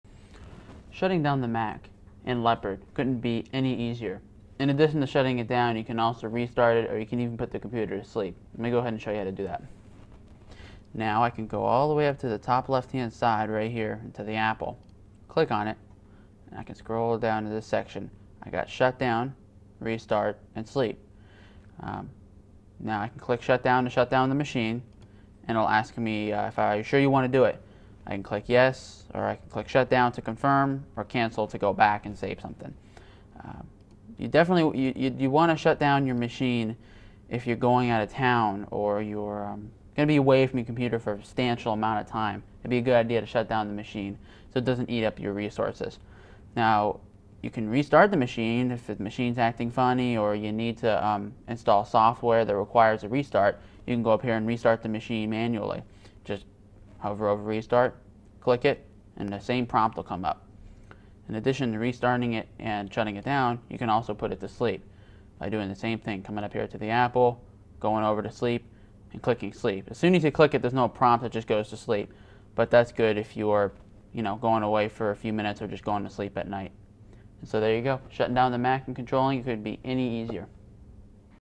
Mac OS X Leopard Overview : Mac OS X Leopard Shutdown
Shutting down, restarting or putting a computer is sleep mode are options in Leopard. Learn how to shutdown a computer in Leopard from an Apple computers specialist in this free computer software video.

Expert: Brandon Karon
Bio: Brandon Karon works for Florida's largest premier Apple reseller. He is also an Apple specialist and has been an Apple sales professional for over two years.